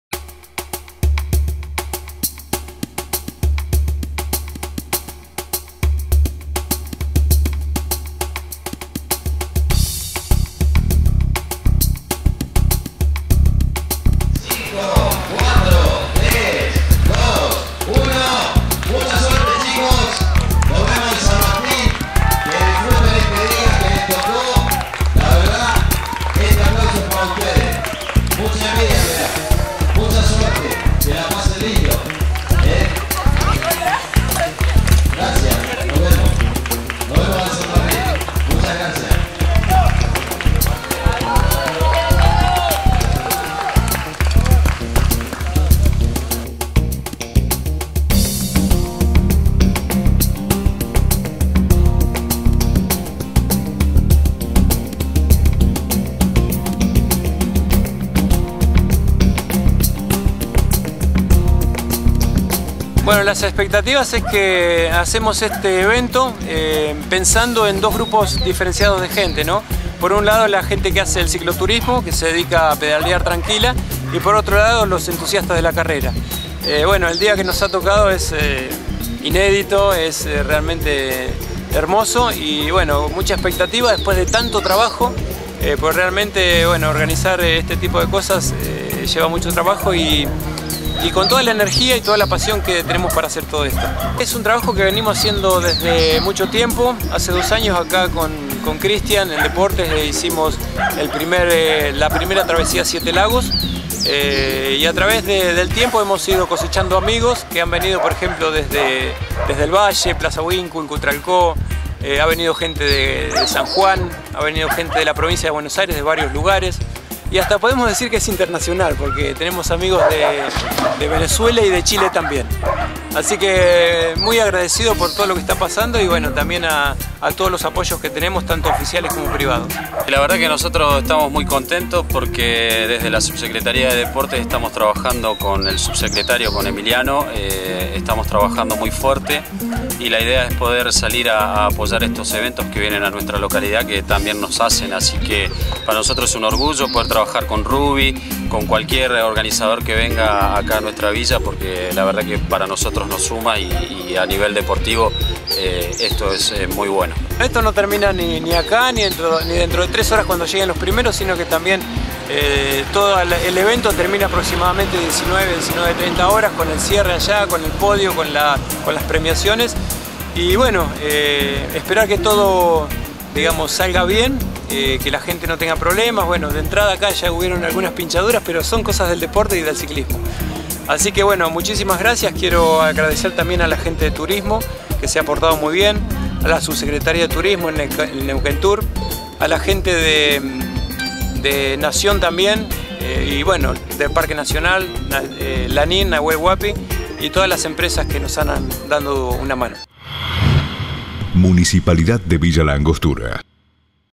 7 Lagos en bicicleta, arranca el DD7L
Con el apoyo de la Subsecretaría de Turismo de la Provincia del Neuquén y el municipio de Villa La Angostura, el domingo 25 de noviembre se pone en marcha la nueva aventura de mountain bike, que une a Villa La Angostura con San Martín de los Andes, a través del majestuoso camino de los 7 Lagos.